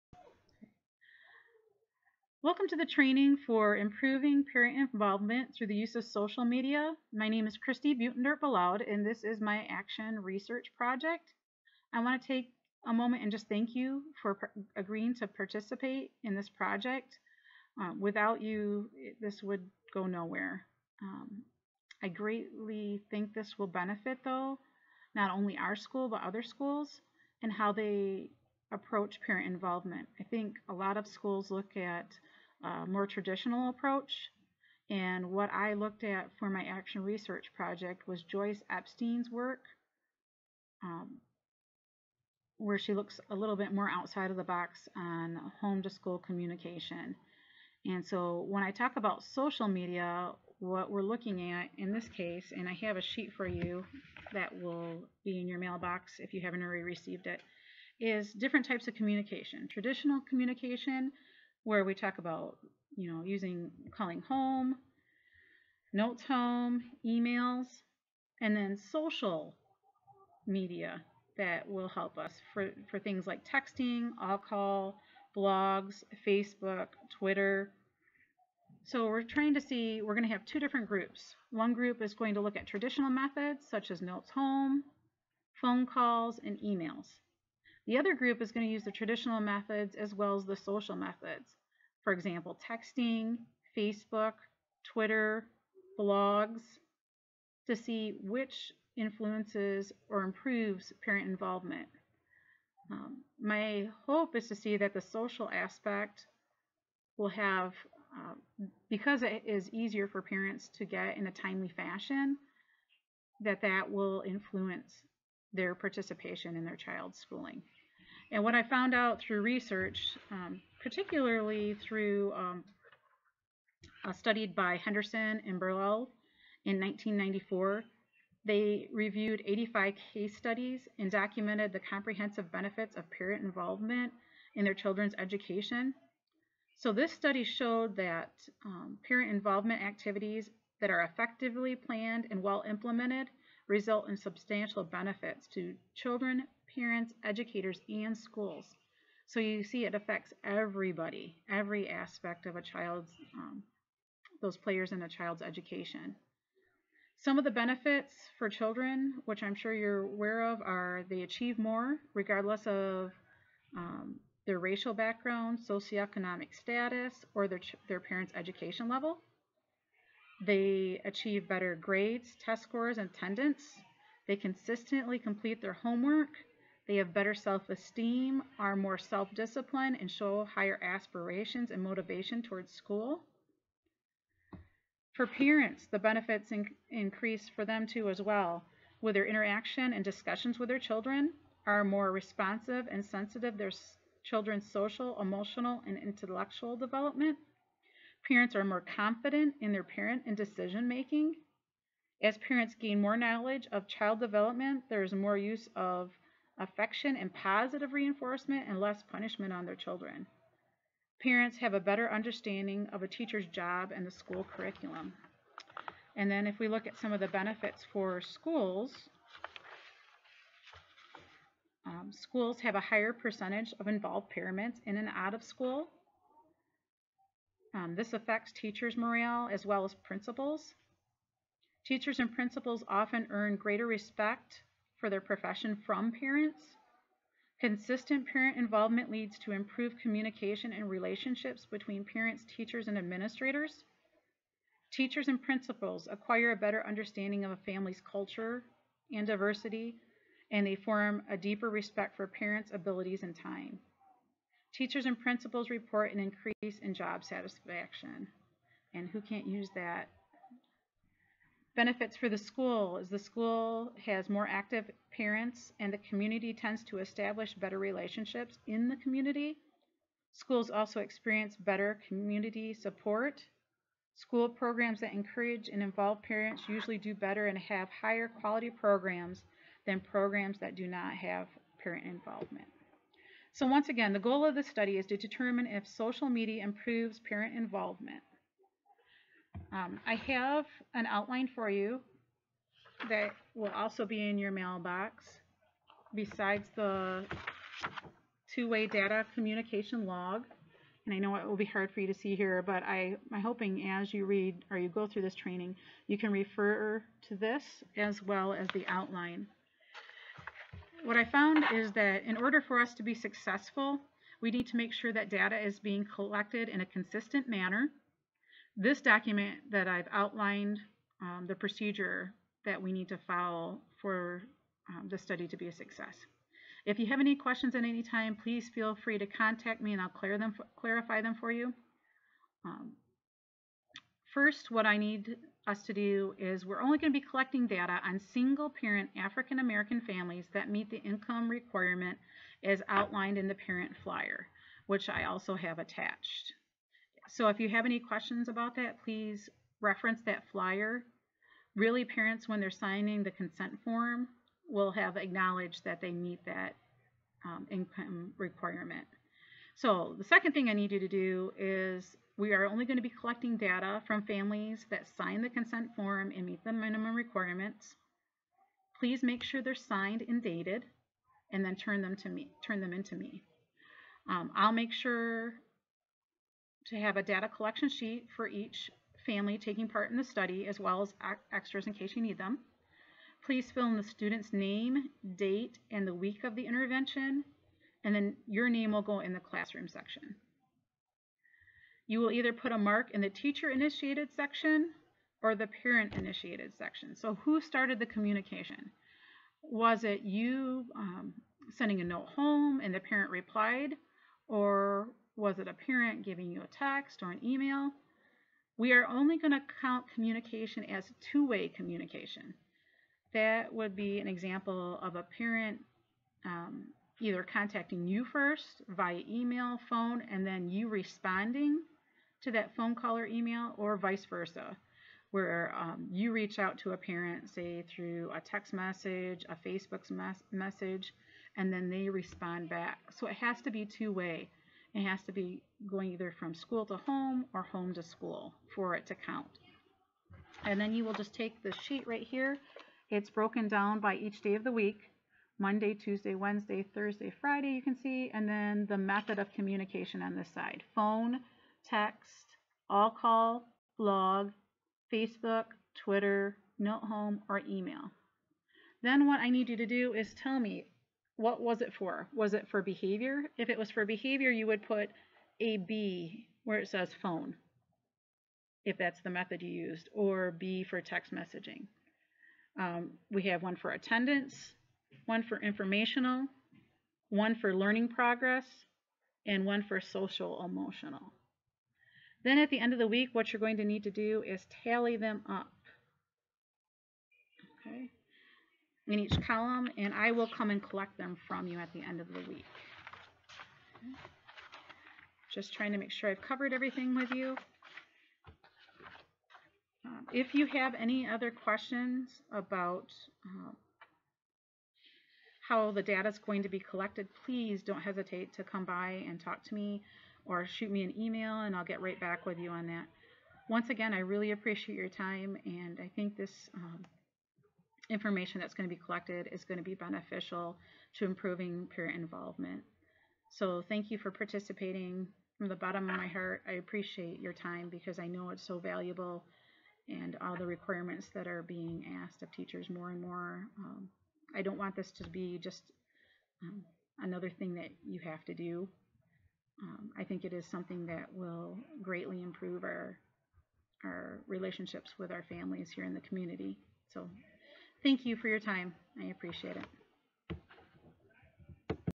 Training Video Parent Involvement Copy
Description: This is the training video for an action research project on improving parent involvement through the use of social media.